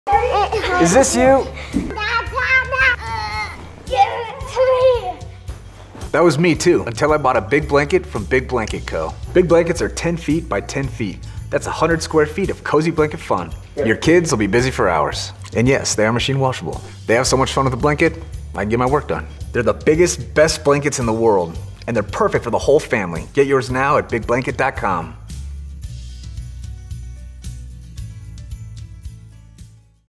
100 Square Feet of Cozy Blanket Fun for Kids - Big Blanket Co
Families love Big Blanket Co 10’ x 10’ blankets because it provides hours of fun -- blanket forts, parachutes, movie nights and more. Big Blankets are the only 10’ x 10’ blanket and made with a specially-formulated polyester and spandex blend you can’t buy anywhere else. So durable it stands up against spills and fights, and it is machine washable.